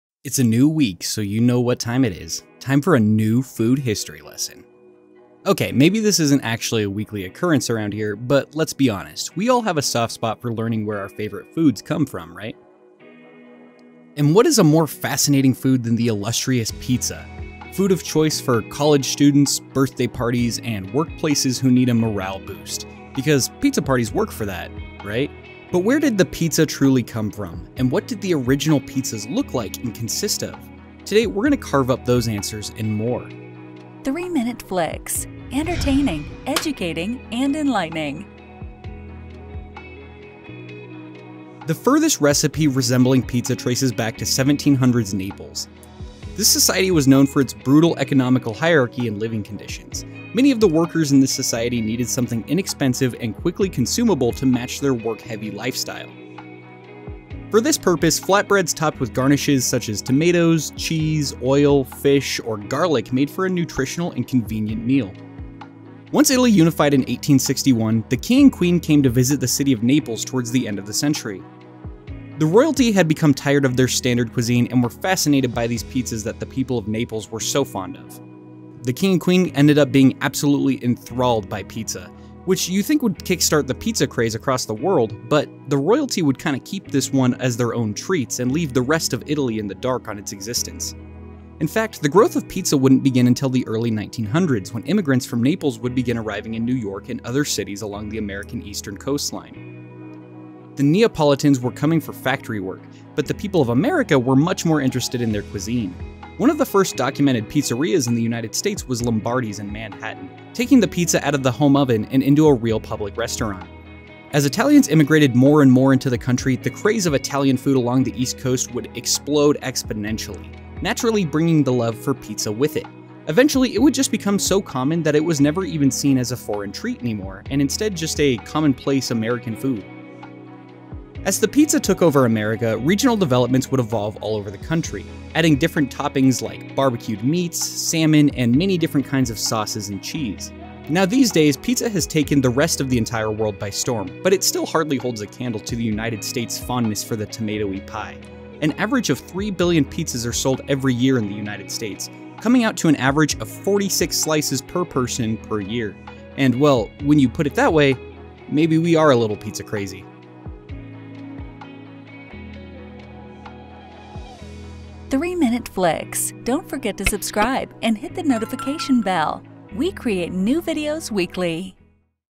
The History of Pizza
Transcript:

It’s a new week, so you know what time it is… Time for a new food history! Okay, maybe this isn’t actually a weekly occurrence but let’s be honest- we all have a soft spot for learning where our favorite foods came from. And what is a more fascinating food than the illustrious pizza. Food of choice for college students, birthday parties, and workplaces who need a morale boost. Because pizza parties work for that, right?
 But where did the Pizza truly come from? And what did the original pizzas look like and consist of? Today, we’re going to carve up those answers and more.
 The furthest recipe resembling pizza traces back to 1700s Naples. This society was known for it’s brutal economical hierarchy and living conditions. Many of the workers of this society needed something inexpensive and quickly consumable to match their work-heavy lifestyle. For this purpose, flatbreads topped with garnishes such as tomatoes, cheese, oil, fish, or garlic made for a nutritional and convenient meal.
 Once Italy unified in 1861, the King and Queen came to visit the city of Naples towards the end of the century. The royalty had become tired of their standard cuisine and were fascinated by these pizzas that the people of Naples were so fond of. The King and Queen were absolutely enthralled by these pizzas, which you’d think would kickstart the Pizza craze across the world, but the royalty would keep this cuisine as their own treats and leave the rest of Italy in the dark on it’s existence.
 In fact, the growth of Pizza wouldn’t begin until the early 1900s when immigrants from Naples would begin arriving in New York and other cities along the American Eastern coastline. The Neapolitans were coming for factory work, but the people of America were much more interested in their cuisine. One of the first documented Pizzerias in the United States was Lombardi’s in Manhattan, taking the Pizza out of the home oven and into a real public restaurant.
 As Italians immigrated more and more into the country, the craze of Italian food along the east coast would explode exponentially, naturally bringing the love for Pizza with it. Eventually, it would become so common that it was no longer even seen as a foreign treat, and instead just considered as common-place as any other American food.
 As the pizza took over America, regional developments would evolve over the country, adding different topics like barbecued meats, salmon, and many different kinds of sauces and cheese.
 These days, Pizza has taken the entire world by storm, but still hardly holds a candle to the United States fondness for the tomato-y pie. An average of 3 billion pizzas are sold every year in the united states, coming out to an average of 46 slices per person per year. And well, when you put it that way… maybe we are a little pizza-crazy.

Coming soon